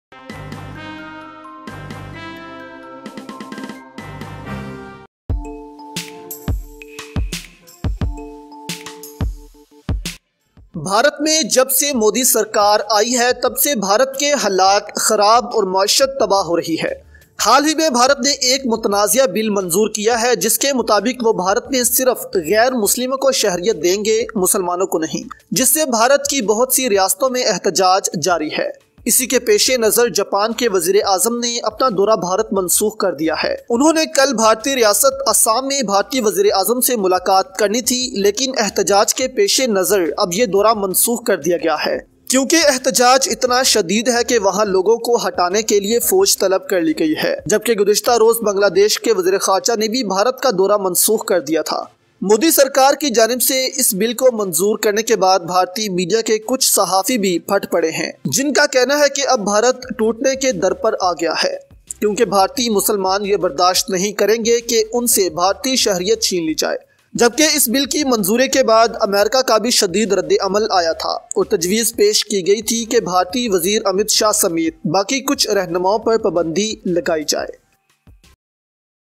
جاپان نے مودی کو بڑا جھٹکا دے دیا || دیکھیں جاپانی وزیراعظم کا فیصلہ
Japan Prime Minister Cancel His Visit Of India || Japan and India Relations 2019